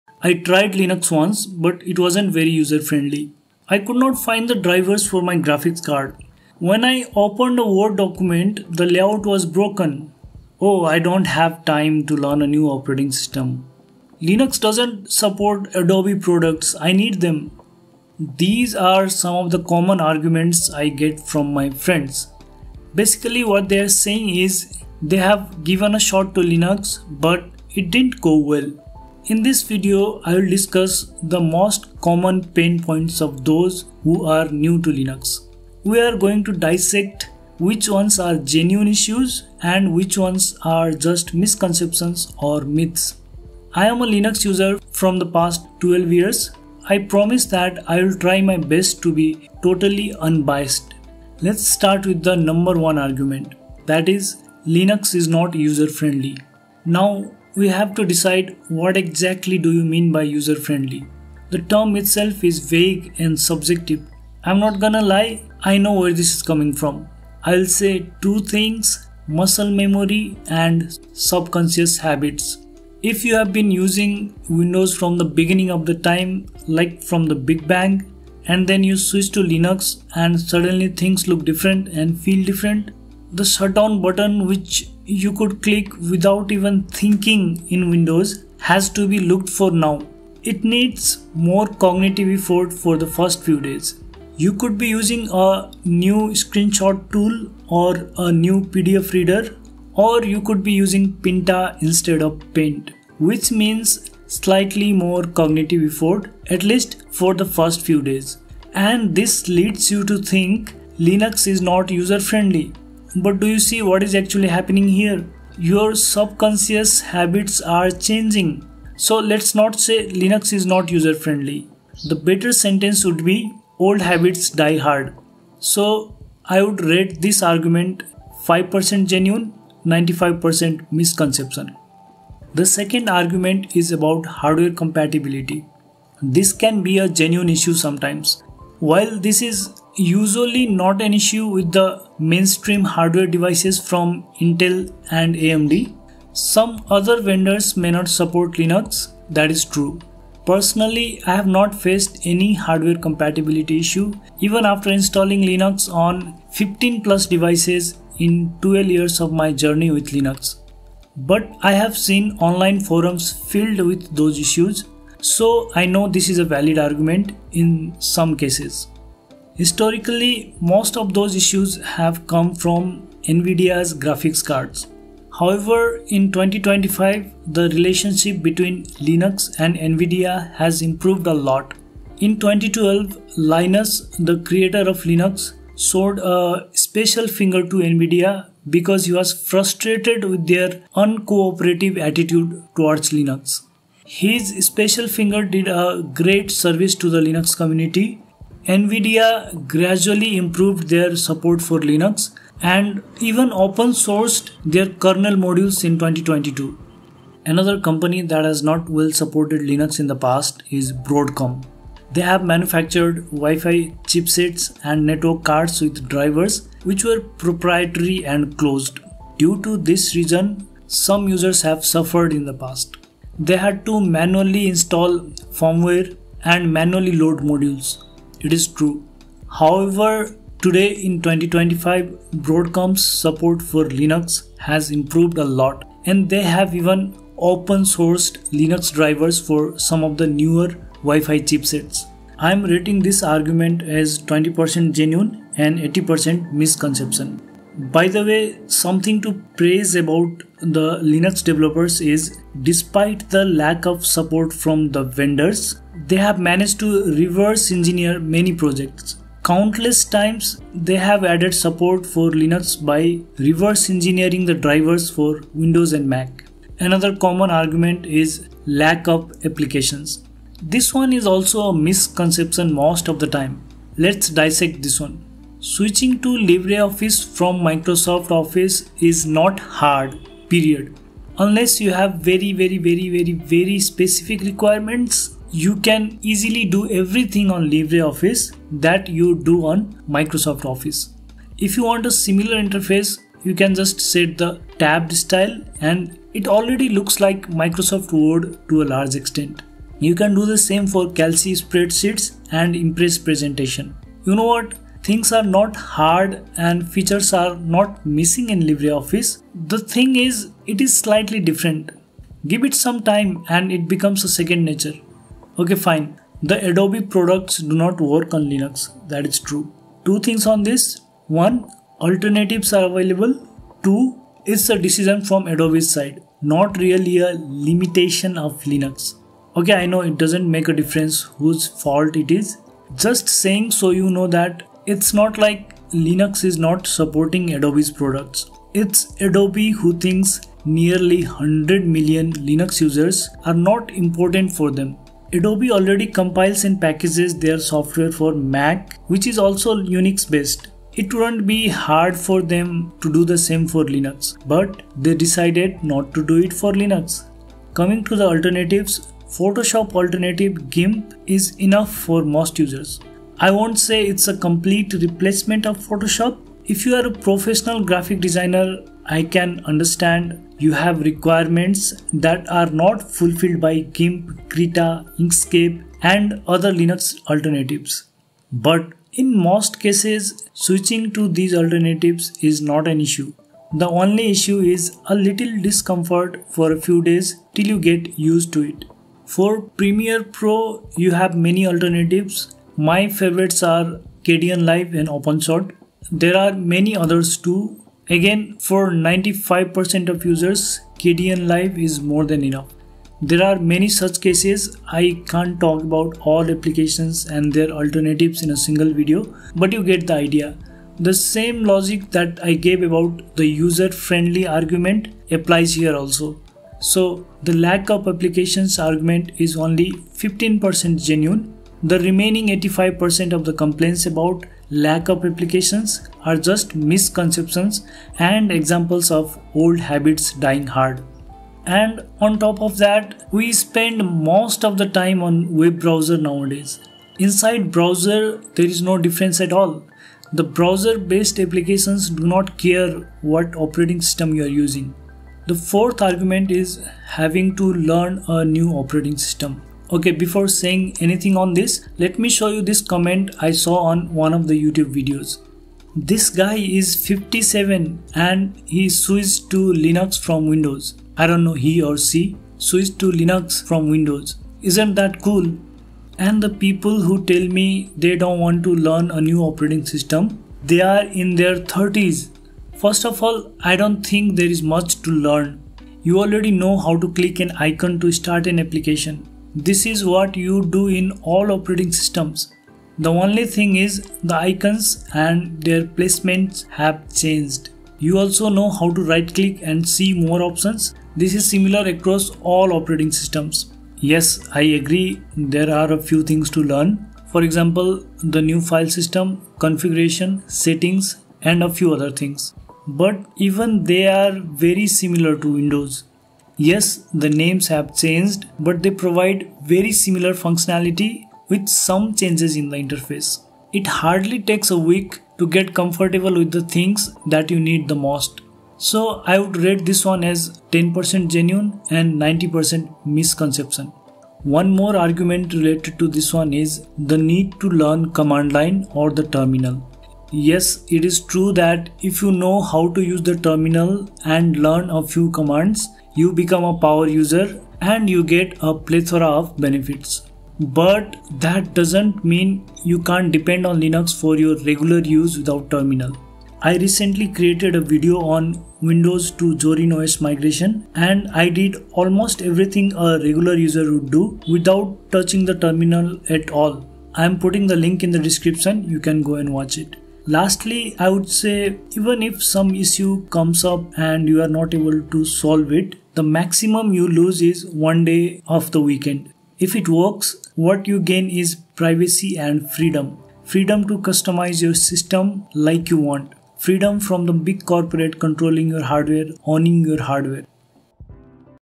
But Linux Does NOT Love Me Back
For those who tried Linux once or are trying to switch to Linux but are facing some difficulties and discomfort.

0:00 Linux pain points
0:53 Linux is not user-friendly
2:14 Hardware Compatibility
4:29 Lack of Applications
7:26 Having to learn a new OS
8:46 Need to learn command line
9:20 Outro